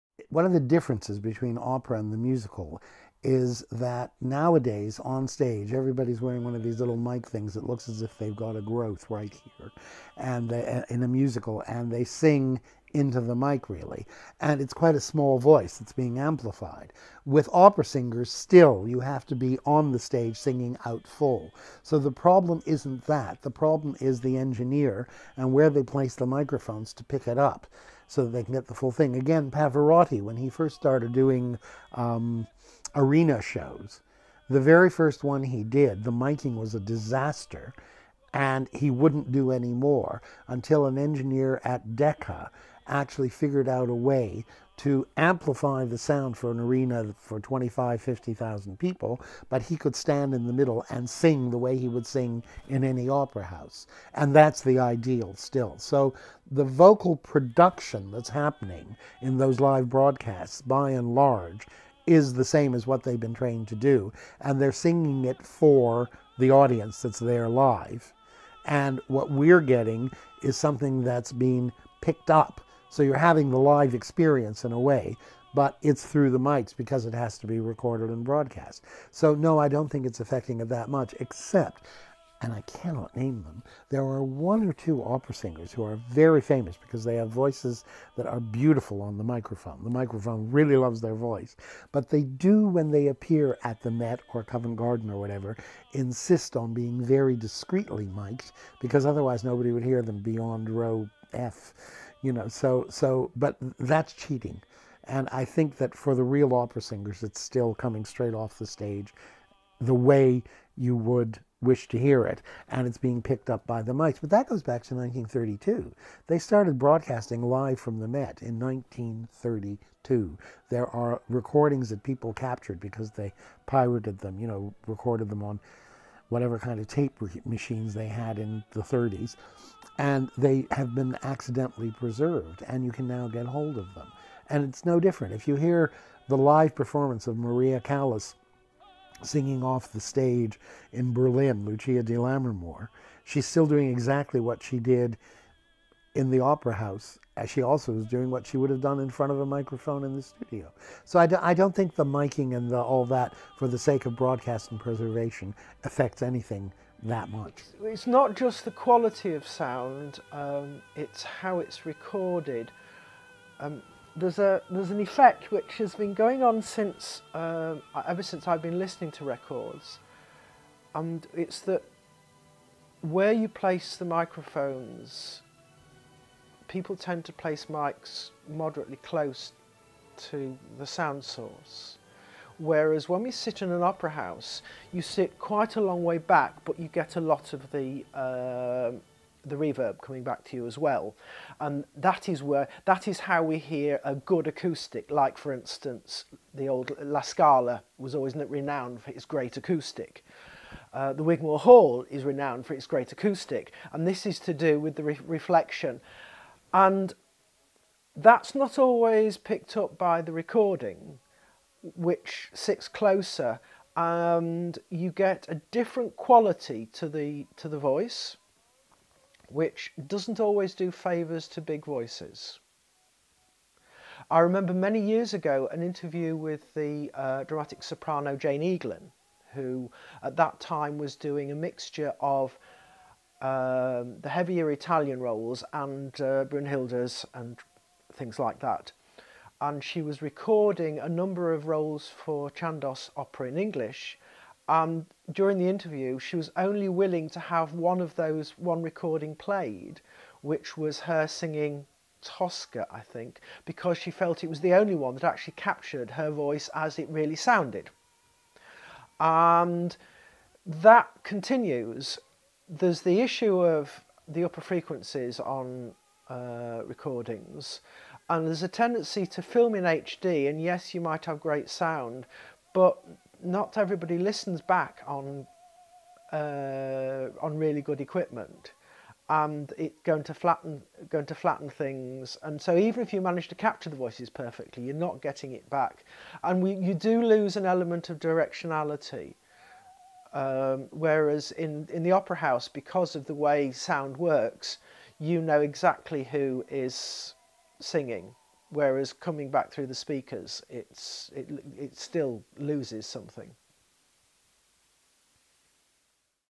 Microphones in Opera
Interviews with experts on opera singers and why they don't use MICs.
Do MICs work for opera singers?  Why or why not? What does this mean for the future of broadcasting opera? ClassicFM presenter Mel Cooper and opera reviewer and composer Robert Hugill discuss this.